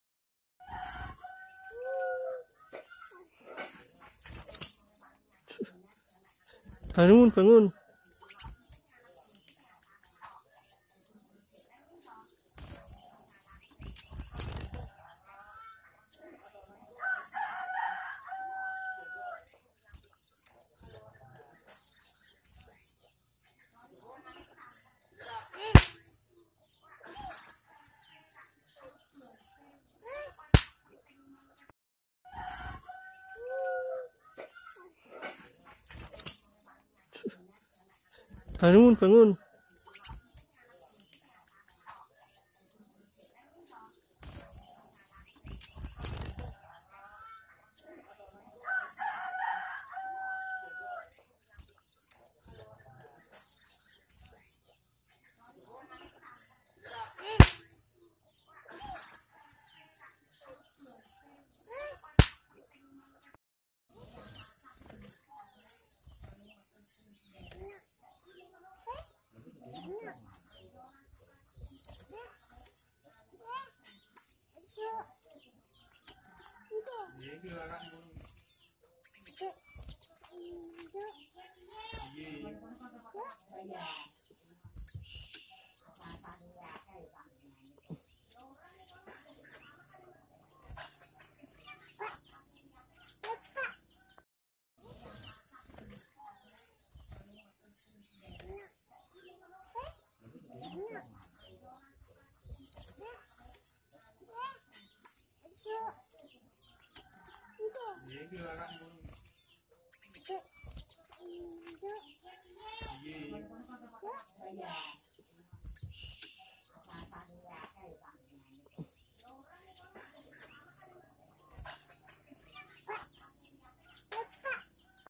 Gouldian finch Gould Amadine Betina siap kawin
Betina gould amadine mulai dewasa, siap untuk dipasangkan dan diternak, bulu sudah mulai lengkap